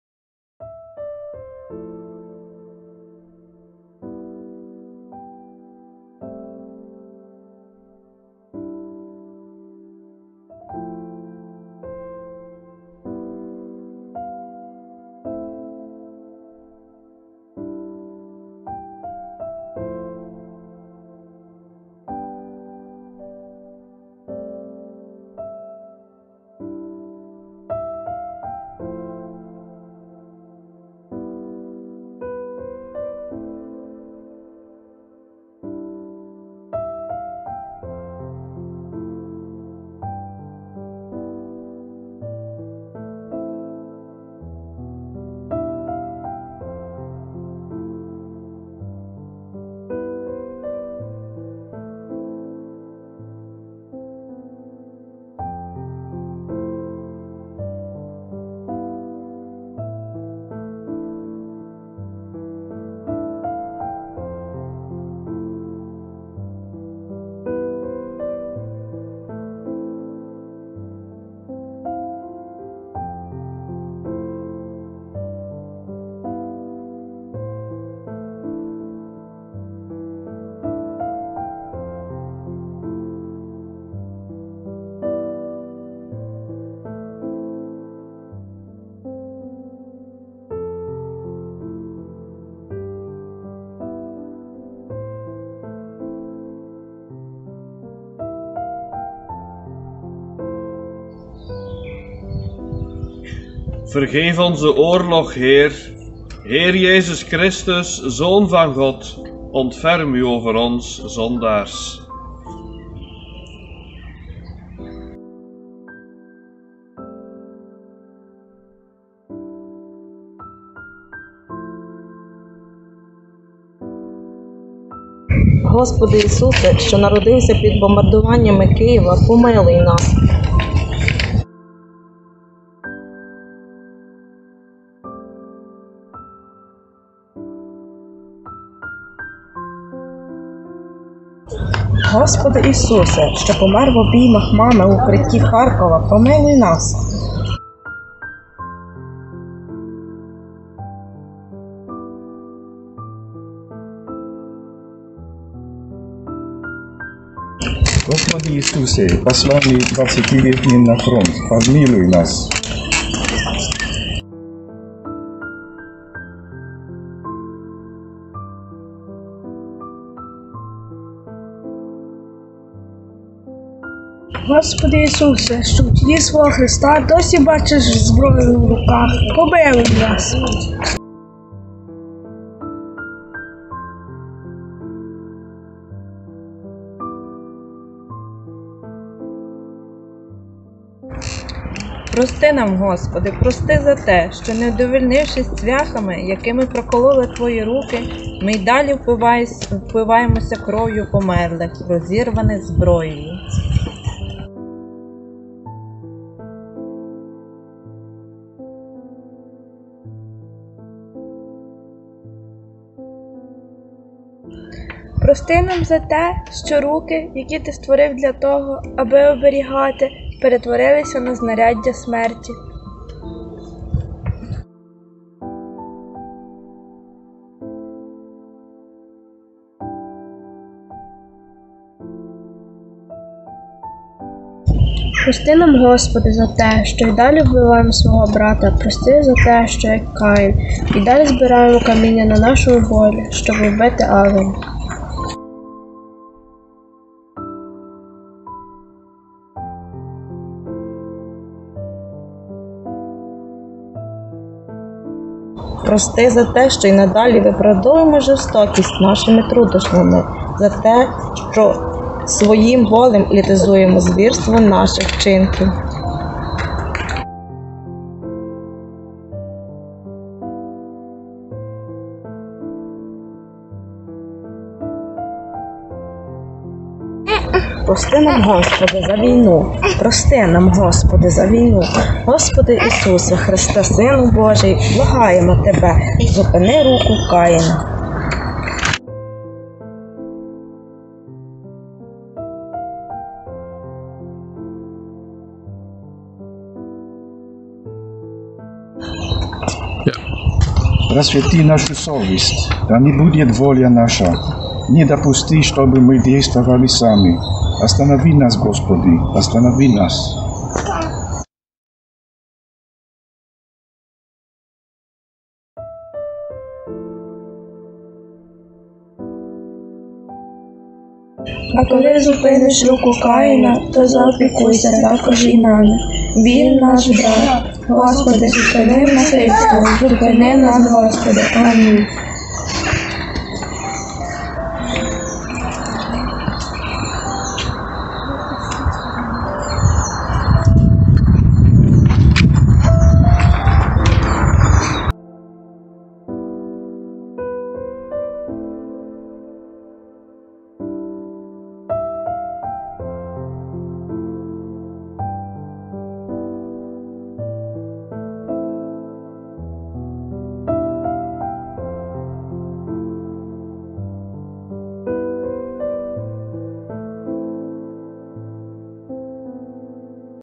Prayer for Peace in Ukraine, Meersel Dreef, with ENGLISH, UKRAINIAN 7 RUSSIAN CAPTION
Prayer of Pope Francis for Peace in Ukraine Молитва Папи Франциска про мир в Україні

The paintings are specially created by Br. Joseph Joyson Palliparambil, OFMCap for the people of Ukraine. He created the paintings in between his cooking of meals for the Ukrainians who found refuge in the Capuchin convent at Meersel Dreef, Belgium. His paintings are reflections of the daily solidarity of the Capuchins of Meersel Dreef, Belgium with the Ukrainians they have welcomed and embraced as their family.

Картини спеціально створені бр. Джозеф Джойсон Палліпарамбіл, OFMCap для народу України. Він створив картини між приготуванням їжі для українців, які знайшли притулок у монастирі капуцинів у Мерсел-Дріф, Бельгія. Його картини є відображенням щоденної солідарності капуцинів з Мерселя Дріфа, Бельгія, з українцями, яких вони вітали та приймали як свою родину.